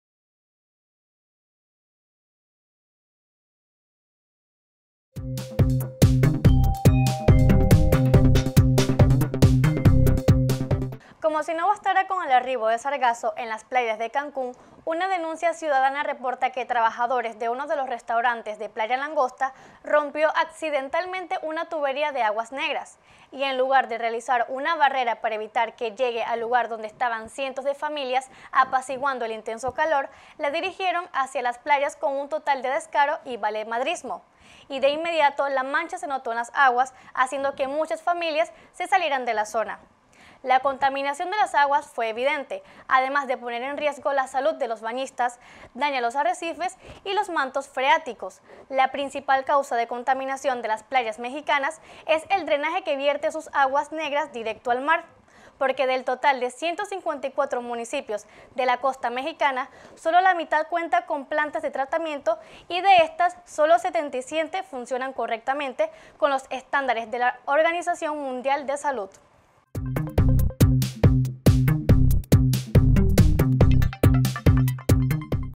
SE LLENA DE “AGUAS NEGRAS” PLAYA LANGOSTA, AL ROMPERSE UNA TUBERÍA
Entérate de las noticias “EN CORTO”…SE LLENA DE “AGUAS NEGRAS” PLAYA LANGOSTA, AL ROMPERSE UNA TUBERÍA...